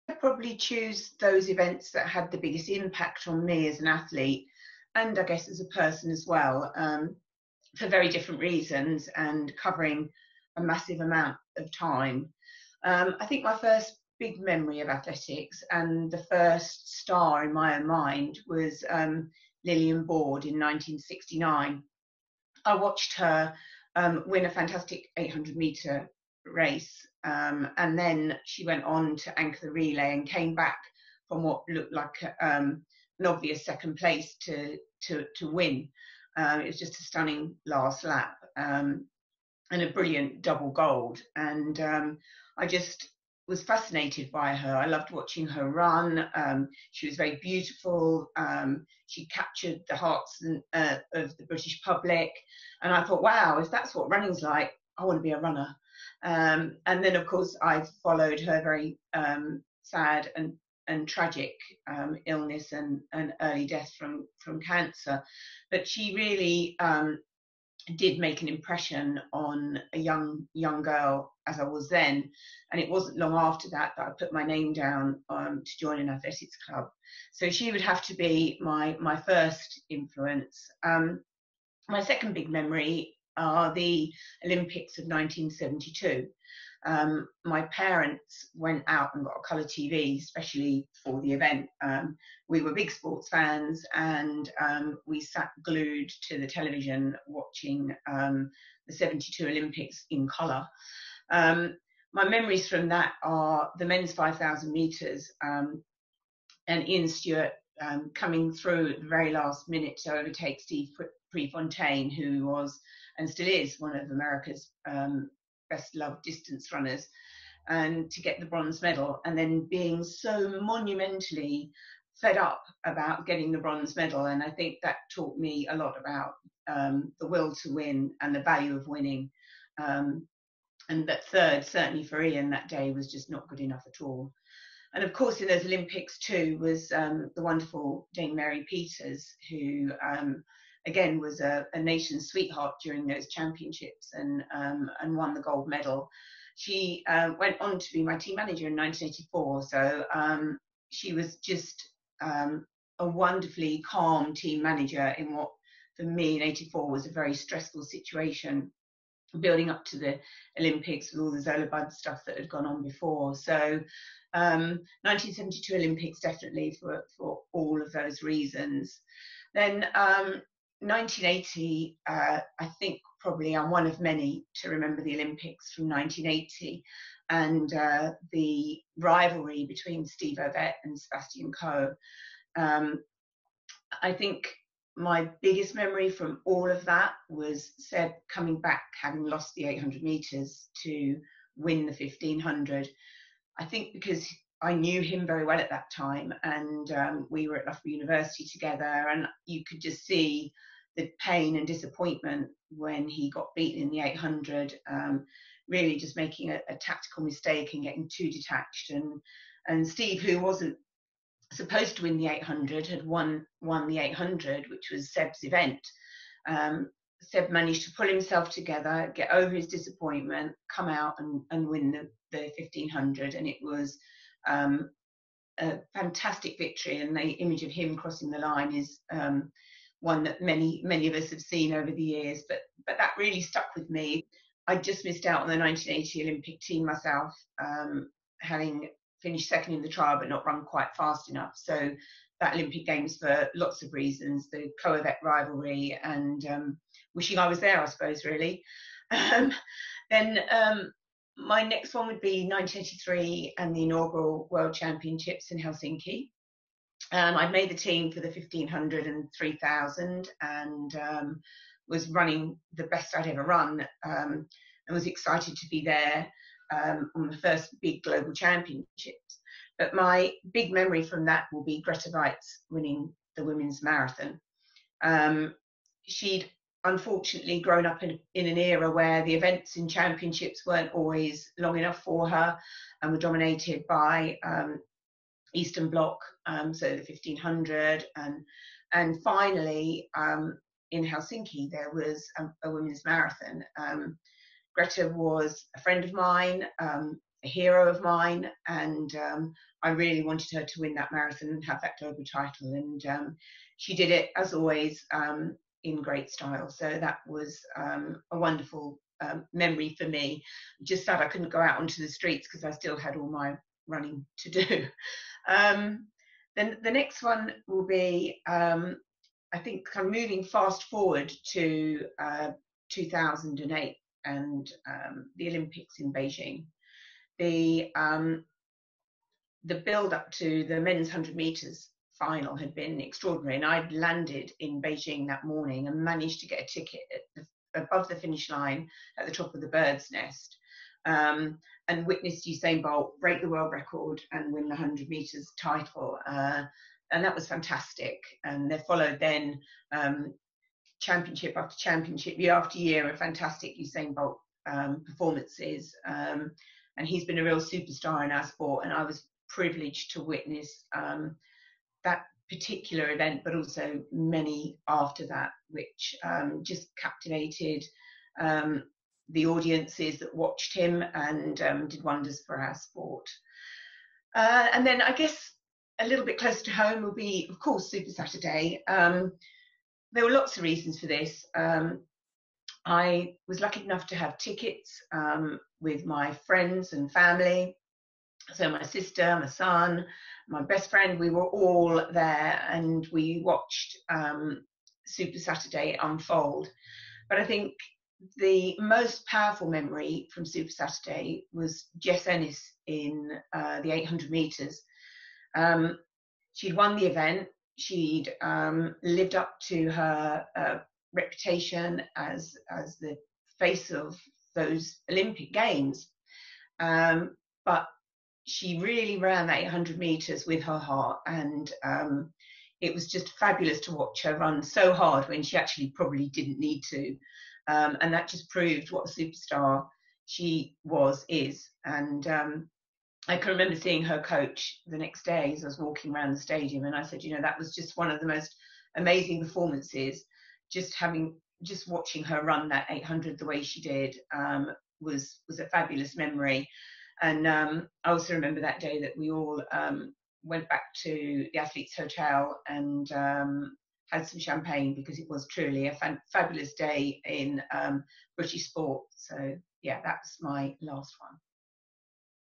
Wendy Sly's fantasy summer of athletics
With little live sporting action to watch right now, as postponements and cancellations continue due to the coronavirus pandemic, many fans are turning to footage of some of their favourite athletics occasions from the past.

Here Britain's 1984 Olympic 3000m silver medallist Wendy Sly shares which athletics moments she would most like to relive during a fantasy season.